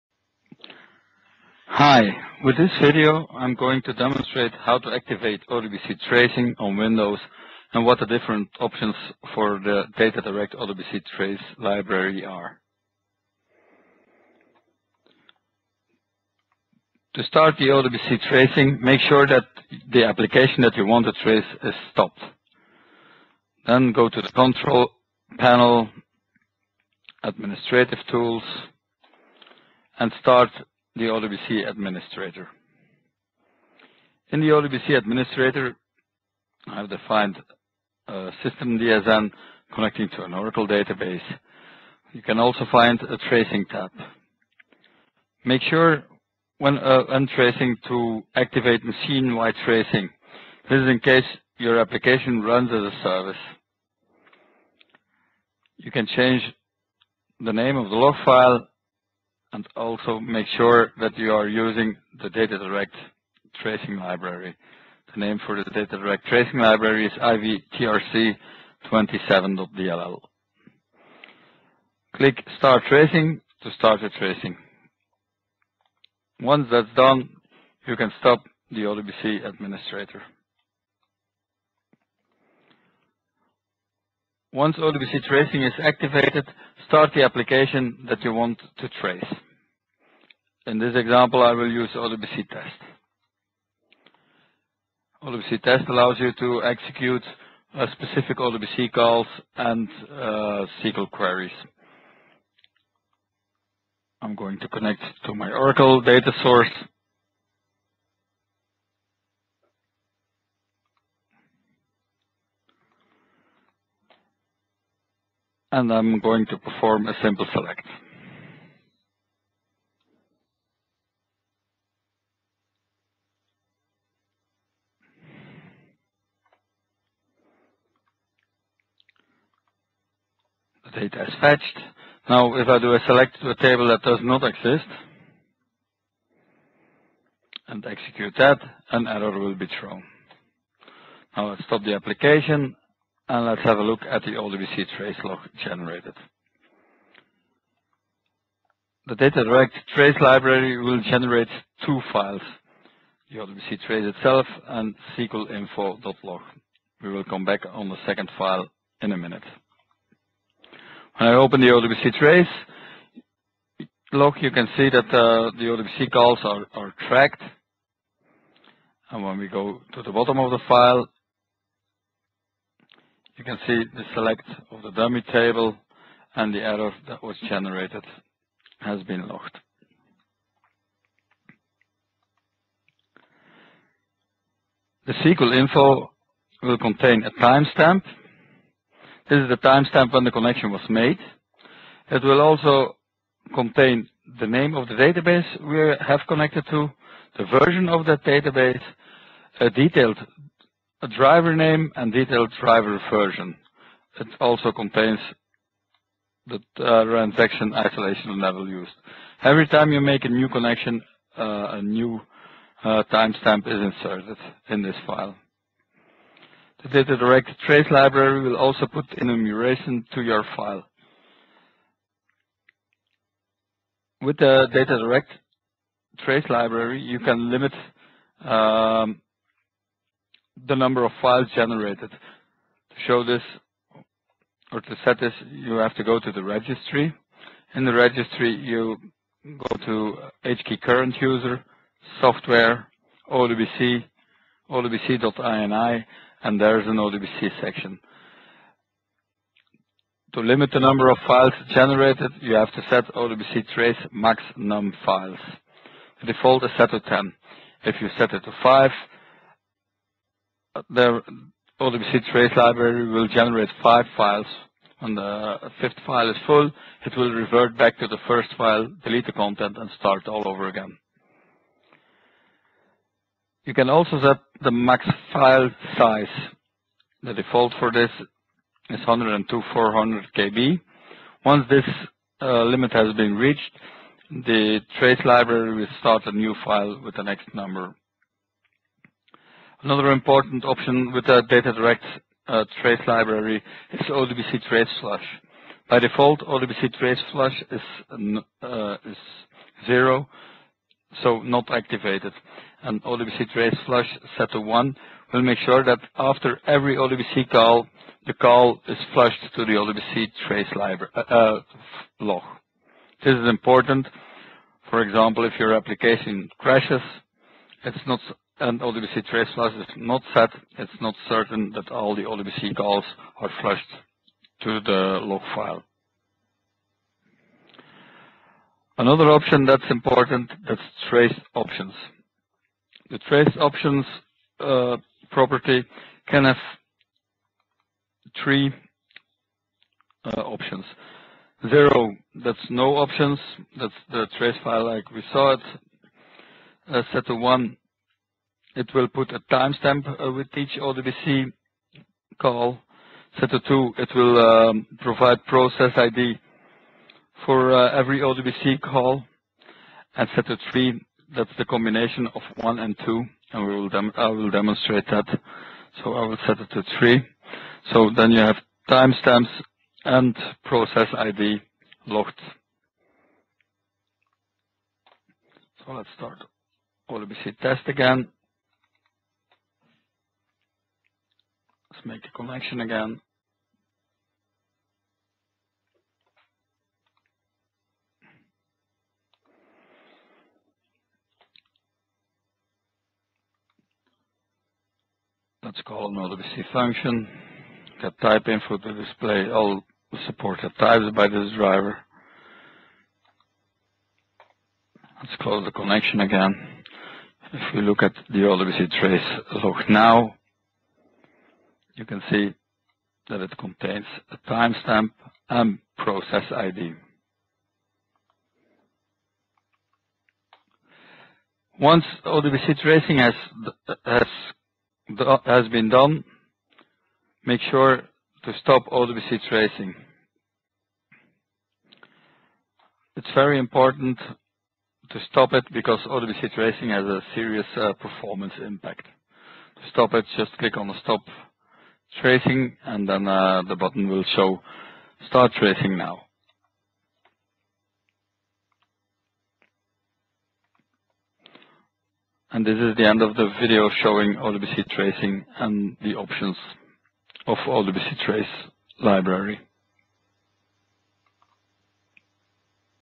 Connect for ODBC Tracing on Windows
This video demonstrates ODBC Tracing on windows and discusses the Trace options.